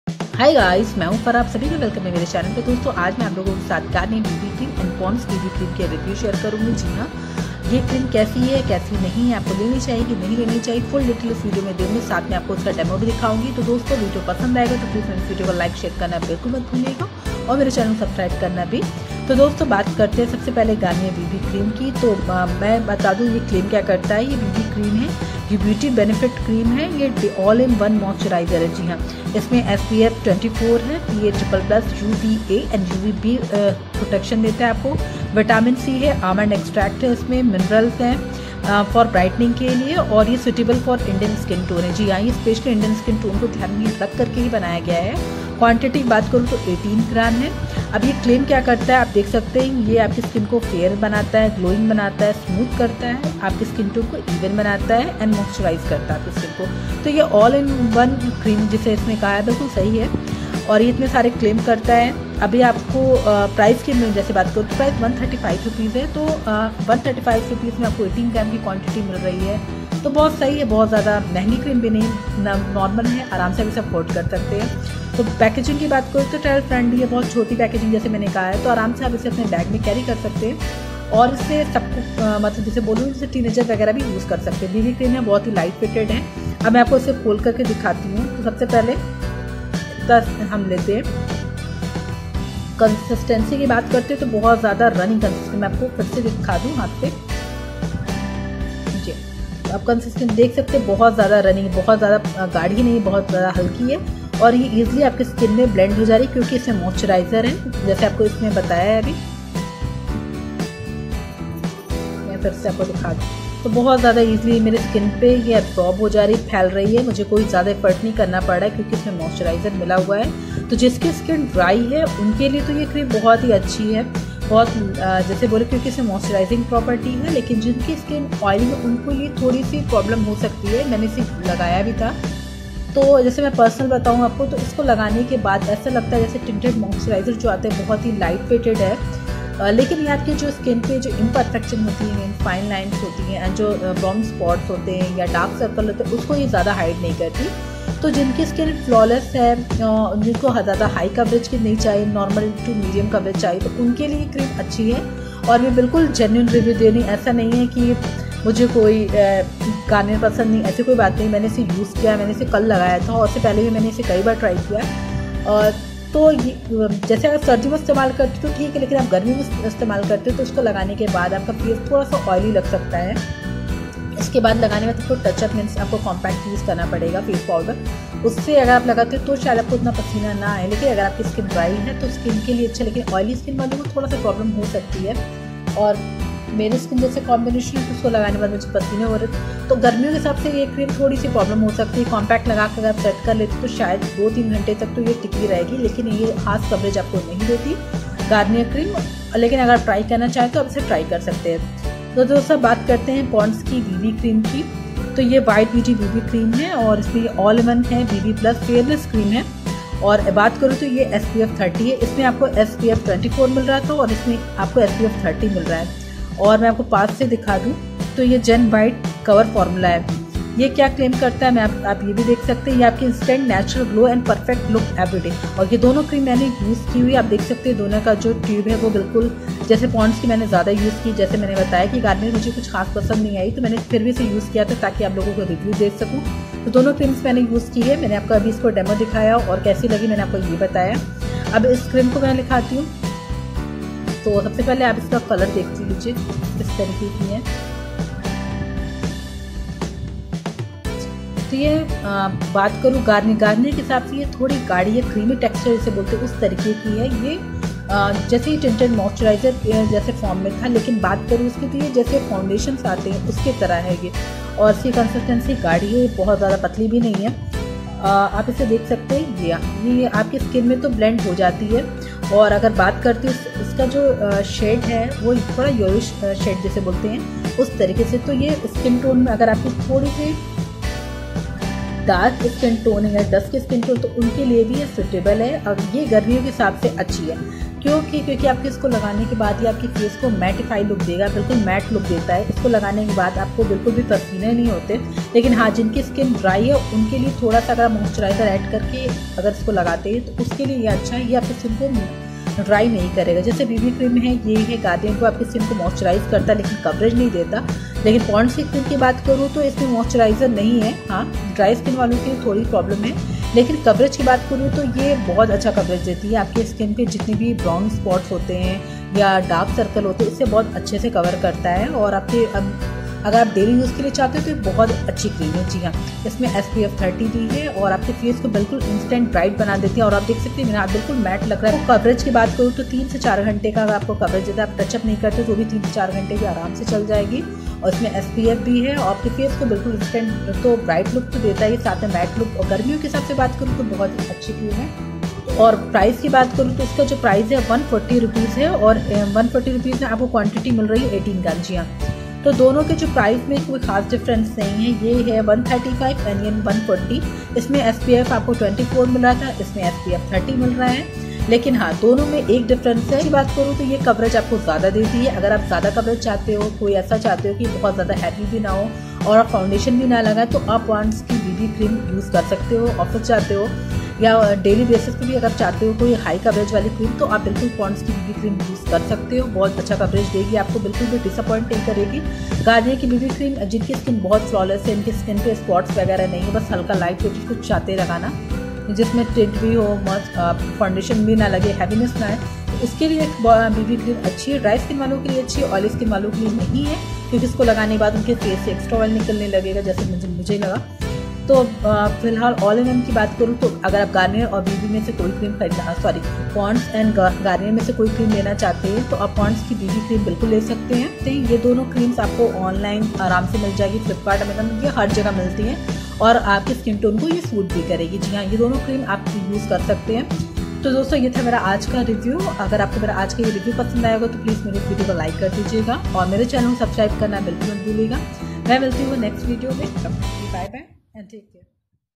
My review on garnier bb cream vs ponds bb cream.
About this video.👇
Dosto aj ke video me ap logo ke sath garnier vs ponds bb cream ke review share krungi.
Thanks.